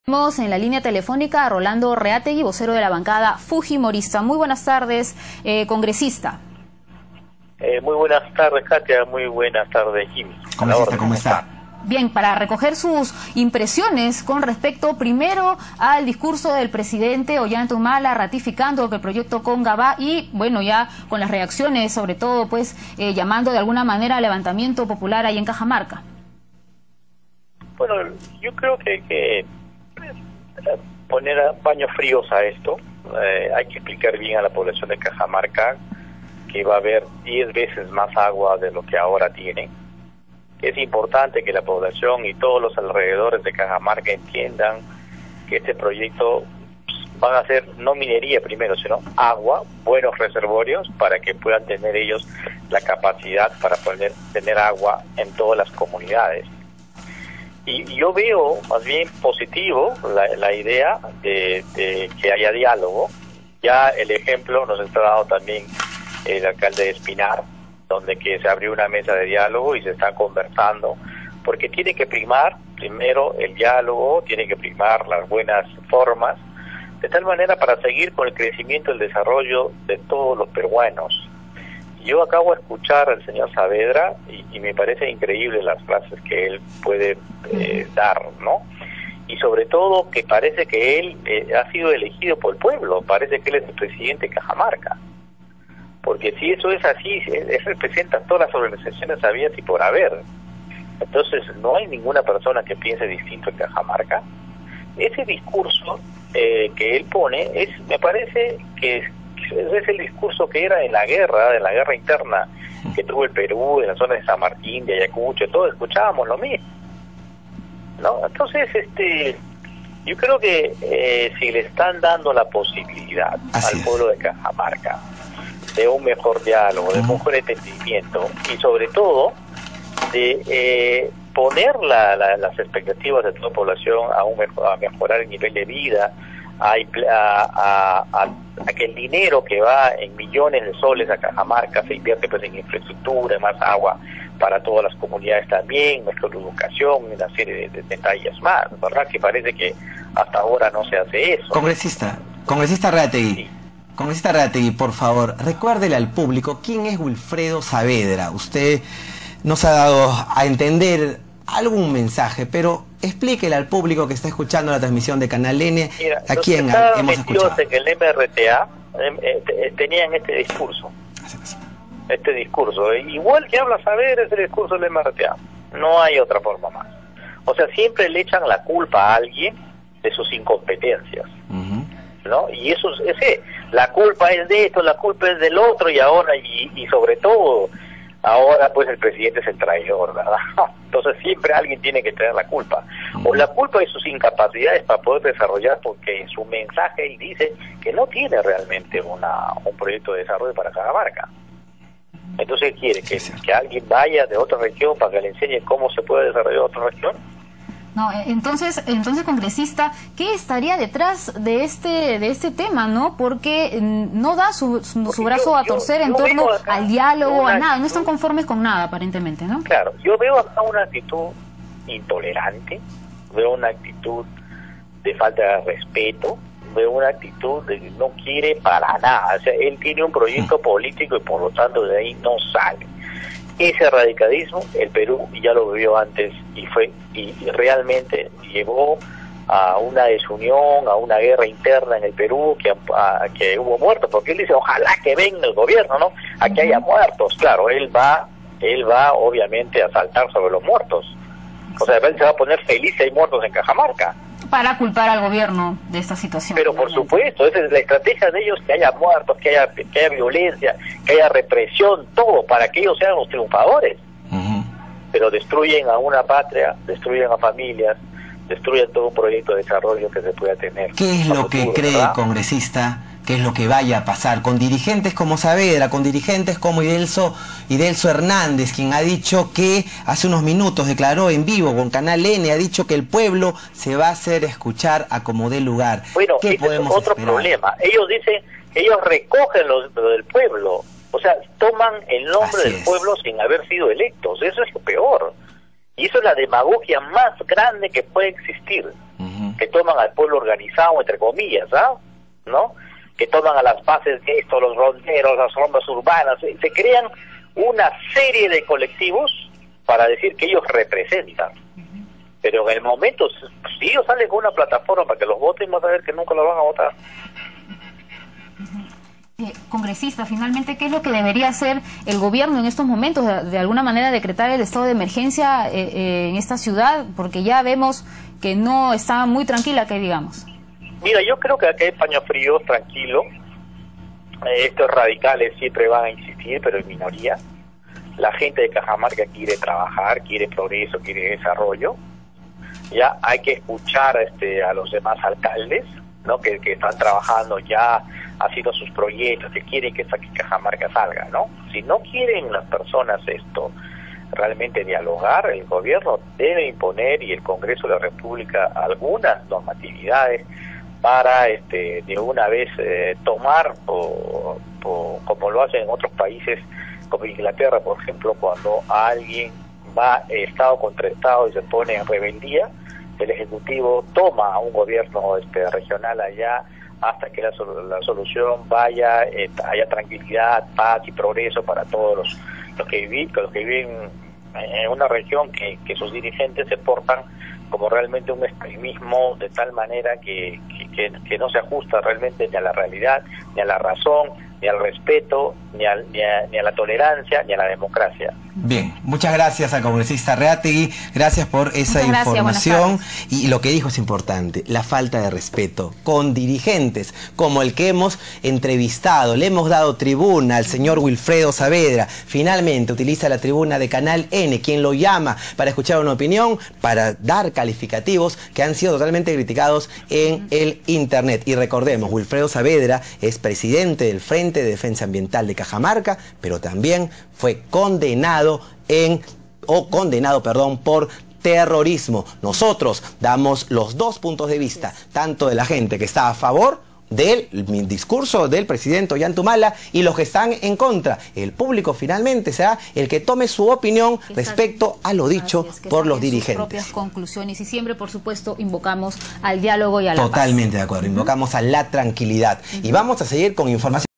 Rolando Reátegui lamentó posición de dirigente cajamarquino
El vocero de la bancada de Fuerza 2011, Rolando Reátegui, dijo que la población de Cajamarca debe saber que ahora dispondrá de una mayor capacidad del recurso hídrico. De otro lado lamentó la posición tajante del presidente del Frente de Defensa Ambiental de Cajamarca, Wilfredo Saavedra.